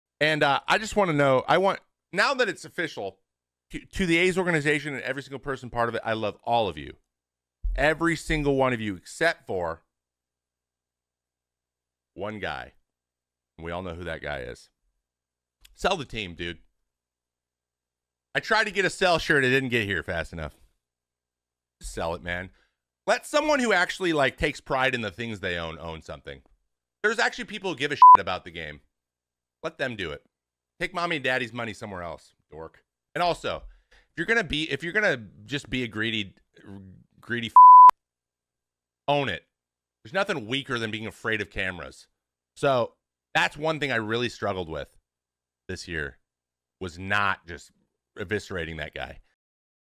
😡 Trevor May shreds ‘greedy’ Oakland A’s owner John Fisher on Twitch stream ⚾
Trevor May may never have to buy a meal or drink in the Bay Area again.

While announcing his retirement Monday night following a nine-year career that ended with two seasons in Oakland, the 34-year-old reliever ripped infamous Athletics owner John Fisher.

“To the A’s organization and every single person part of it, I love all of you, every single one of you, except for one guy and we all know who that guy is,” May said during a stream on Twitch.